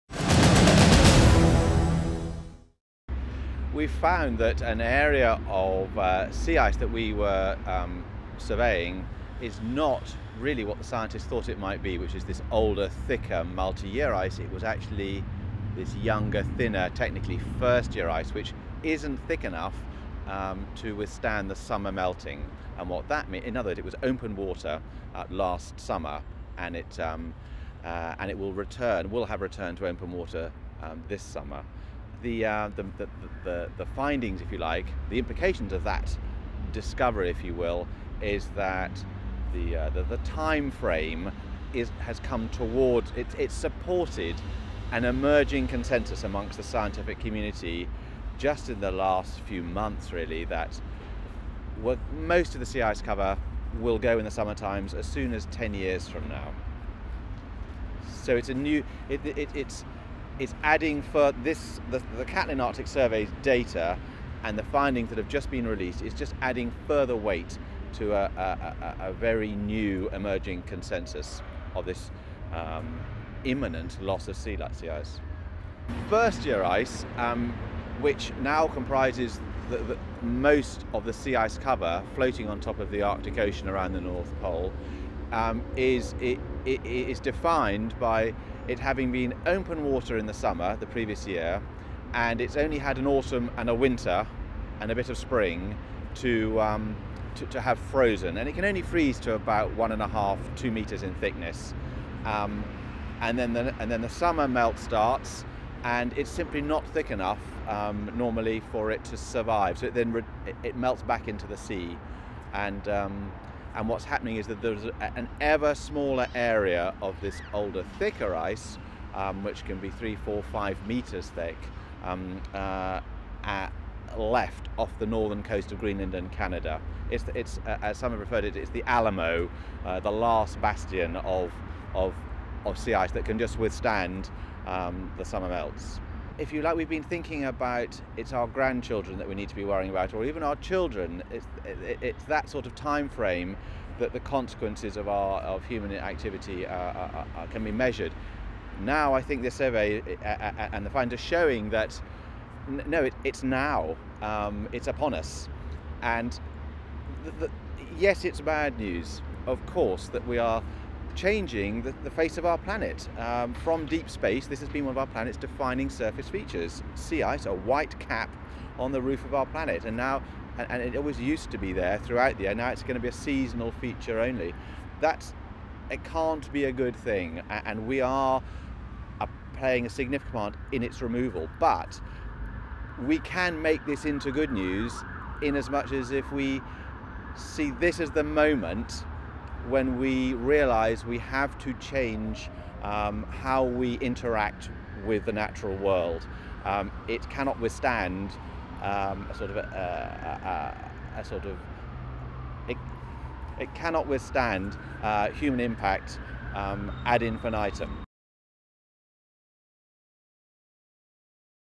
Pen Hadow on what he saw in the Arctic
After leading the Catlin Arctic Survey, explorer Pen Hadow says it's likely the Arctic will be free of summer sea ice within a decade.